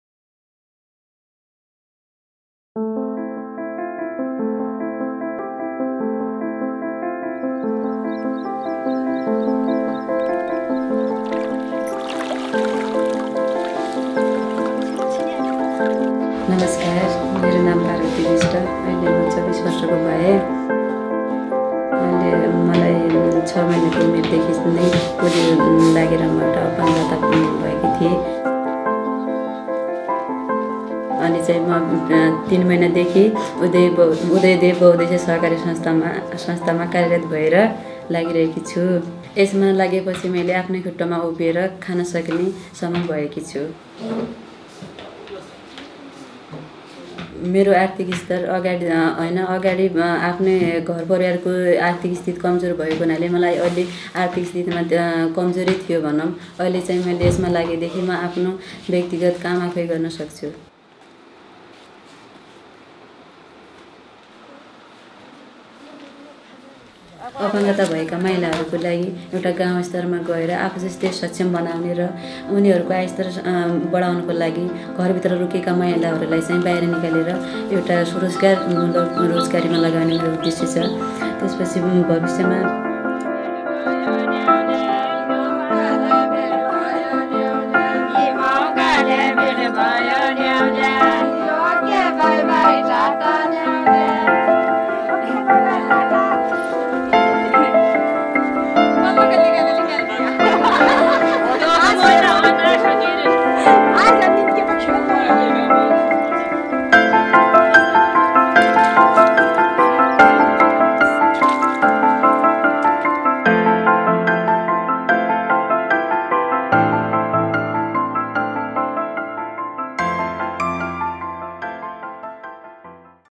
La nouvelle vie de Parbati Bista - Népal
Parbati Bista, 24 ans, travaille dans une institution financière grâce au travail de plaidoyer effectué dans la région. Elle est en mesure aujourd'hui d'aider à son tour sa communauté.